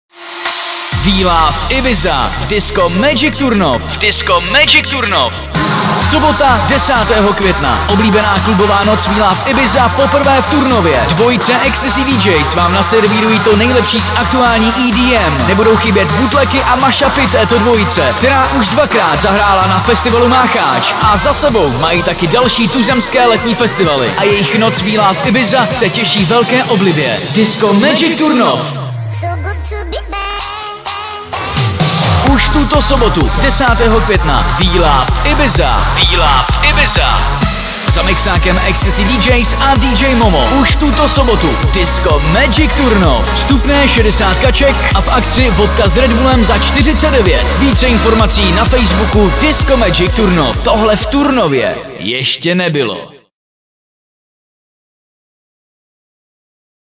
pozvánka MAGIC TURNOV / WE LOVE IBIZA / 10/5/2014
WE ♥ IBIZA / vstup 60,-
Klubová noc WE ♥ IBIZA je tady! Dva čeští energičtí DJs - Jirka Vondra & Atomic - společně za mixem pod názvem EXTASY DJs - si vás vezmou pořádně do parády! Doprovázet za mixem je bude talentovaný DJ Momo. Neváhej a doraz na tuhle výjimečnou párty!
Už v sobotu 10.května v DISCO MAGIC TURNOV!!!
AKČNÍ DRINK pro tento večer..: Vodka & Red Bull 49,-

♥♥♥ EXTASY DJs & DJ Momo Live ve vašem klubu! ♥♥♥